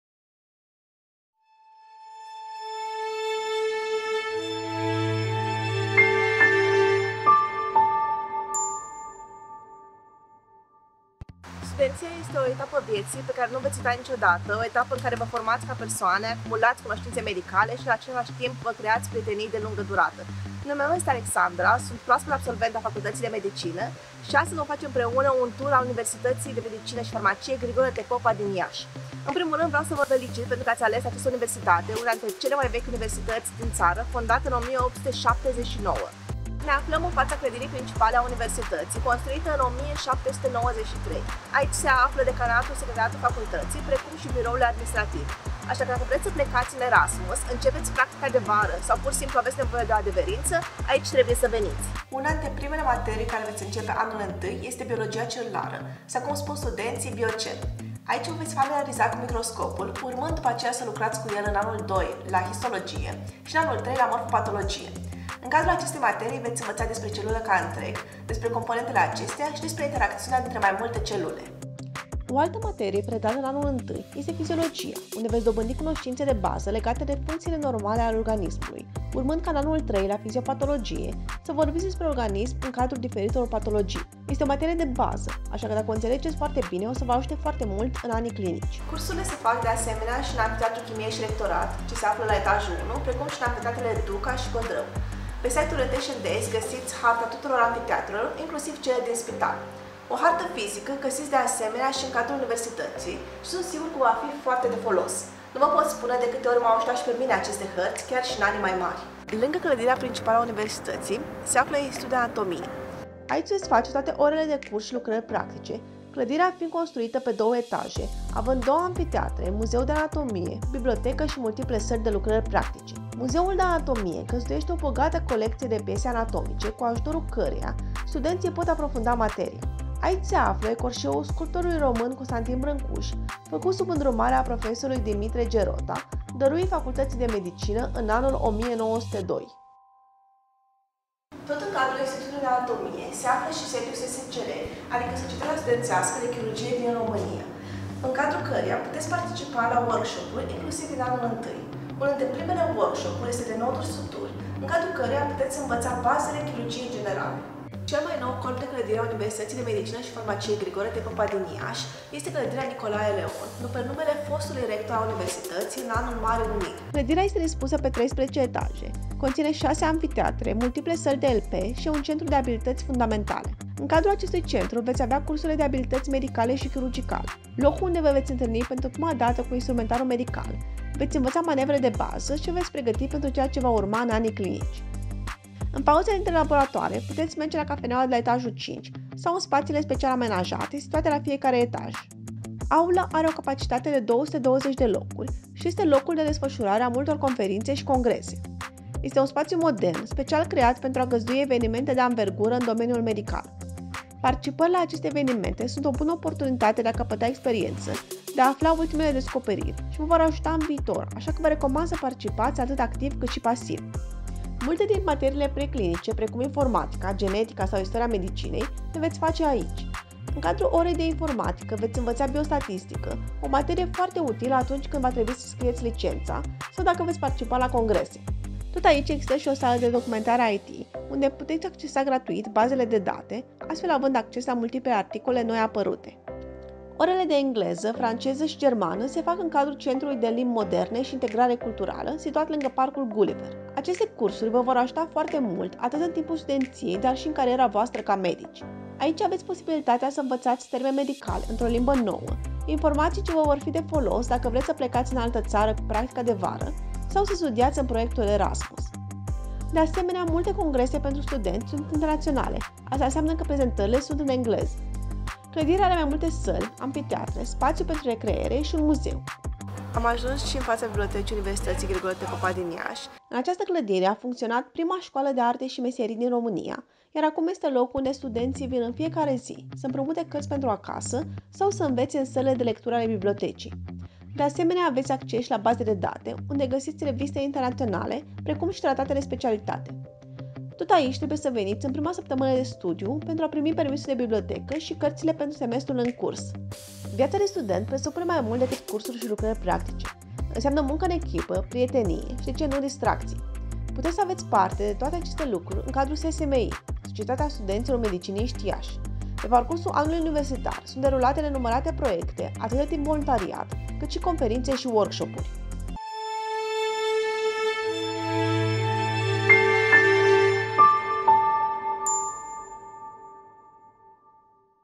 Visite virtuelle :: U.M.F. « Grigore T. Popa » Iasi :: Faculté de médecine
Visite virtuelle dans les bâtiments de l’Université de médecine et de pharmacie « Grigore T. Popa » Iasi - Faculté de Médecine (FR)